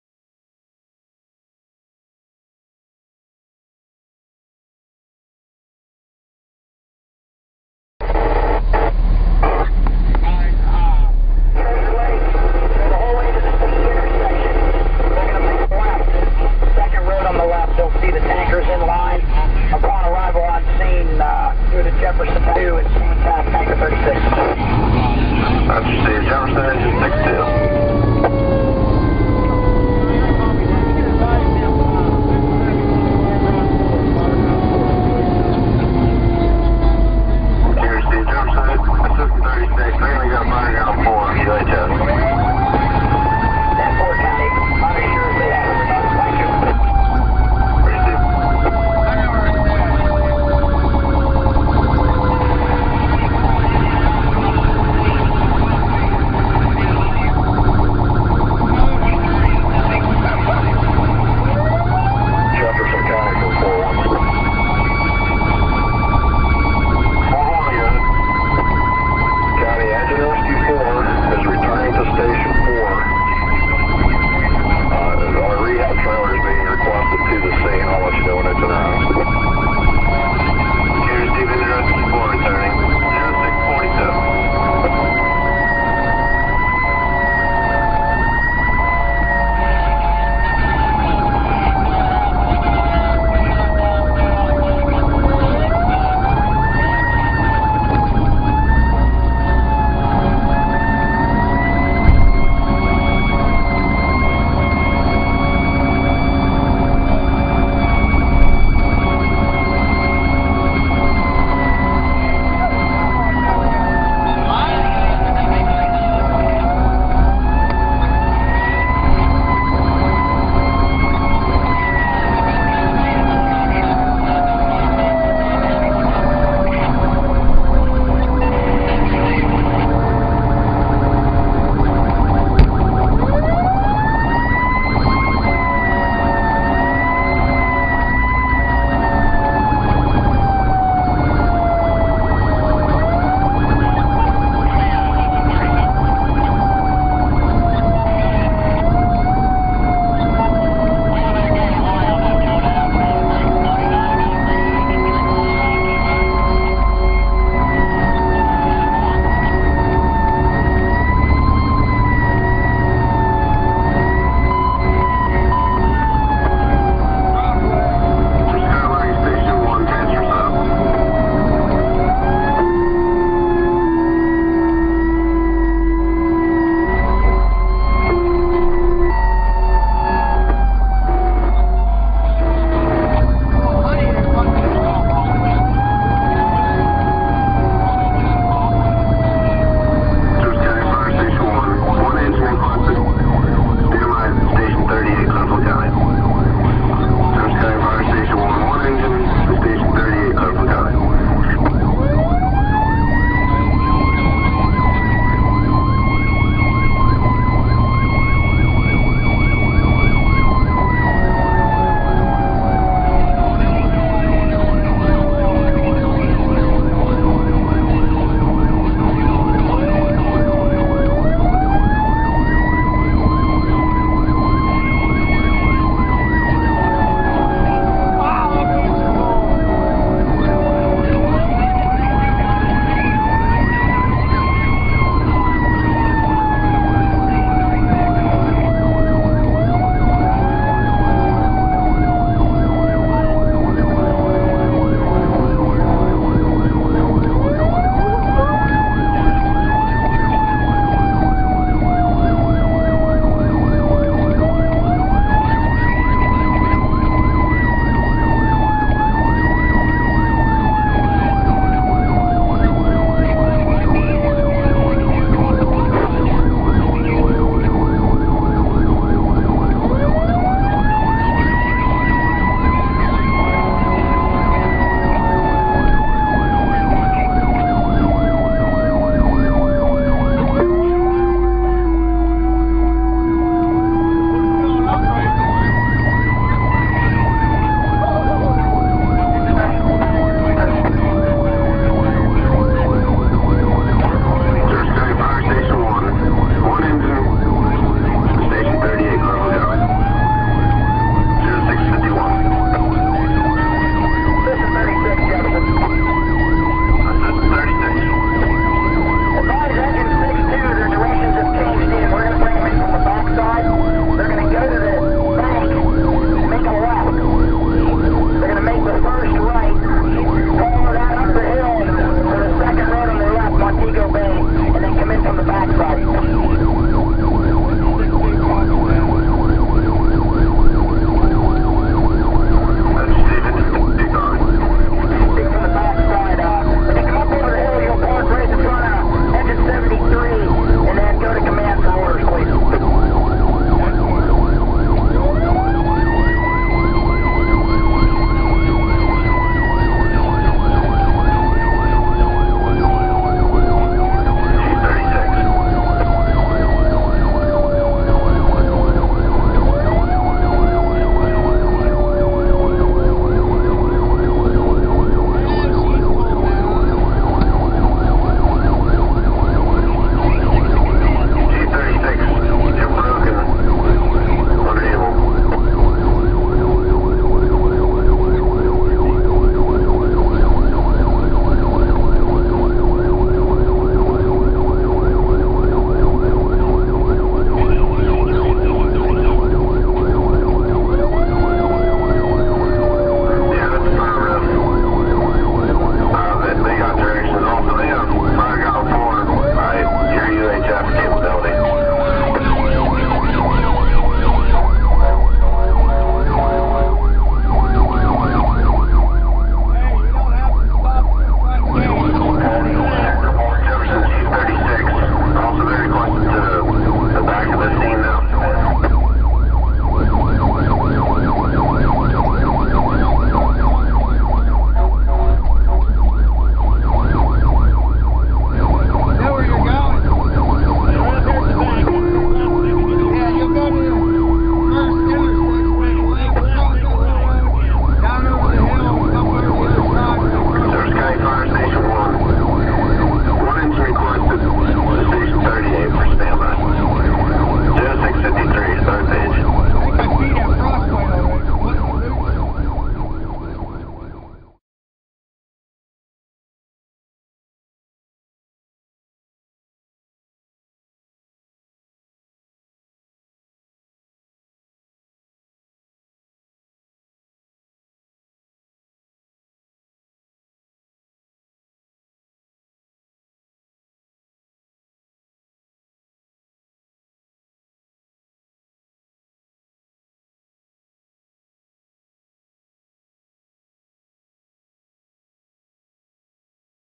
3 Alarm Structure Fire, Clearfield Fire Box 35-A 2-6-2007, - Reynoldsville Fire
Reynoldsville FD Fire Box 35-A Structure Fire Treasure Lake 2-6-2007   Responding From Clearfield County PA Company 36 To The Scene. Temperature Is  -5 to -11 Degrees. Crews on Scene And On Stand By For Nearly 8 Hours.